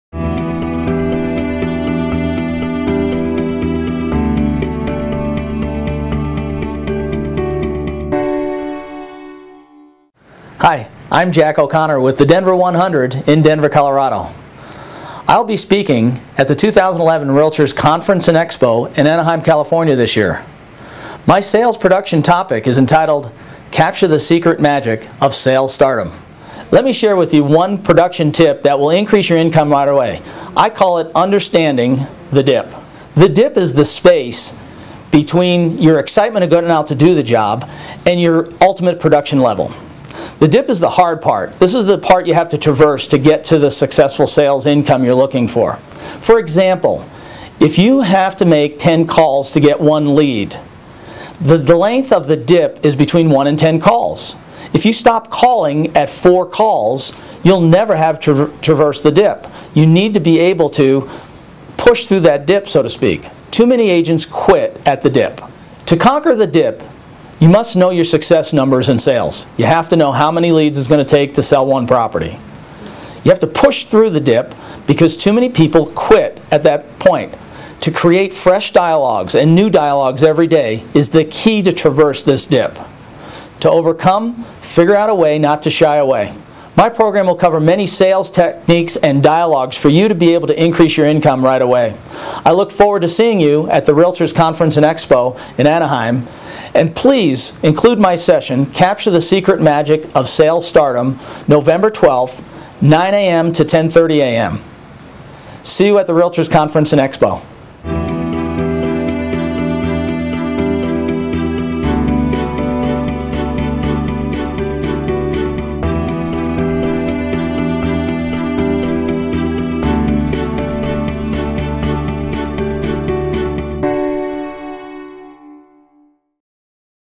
Jack O'Connor - Capture the Secret Magic of Sales Stardom
Video for Innovative Sales Class dialogue to use by Jack OConnor:

Hi, I am Jack OConnor, with The Denver 100 in Denver, Colorado.  I will be speaking at the 2011 REALTORS Conference and Expo in Anaheim. CA. My sales production topic presentation is entitled, Capture the Secret of Sales Stardom.

Let me share with you one production tip that is assured to increase your income right away.   I call this tip the Understand the Dip

The Dip is the space between your personal excitement of getting out there to do your job and getting production.  The Dip is the hard part of sales and you have to learn to traverse the Dip to increase production.  For example, if it takes 10 calls to get one lead, the Dip length is between 1 and 10 calls.  If you stop talking to people at 4 calls for whatever your reason, you do not cross the Dip to the production you are searching for in your business.  Too many agents are limited by The Dip.

To conquer The Dip you must know your personal success numbers to achieve the production.   Then you must have the dialogues that are creative and fresh to allow you confidence to be visible enough each day.   To overcome The Dip, you must push your way through the dip, not shy away.

My program will cover many sales tips and examples on techniques Top Producers use to develop an agent to move towards Stardom.  I look forward to seeing you in Anaheim and please be sure to include my session Capture the Secret Magic of Sales Stardom! on, Saturday, November 12 from 9:00 AM to 10:30 AM on your schedule.

See you at the REALTORS Conference & Expo!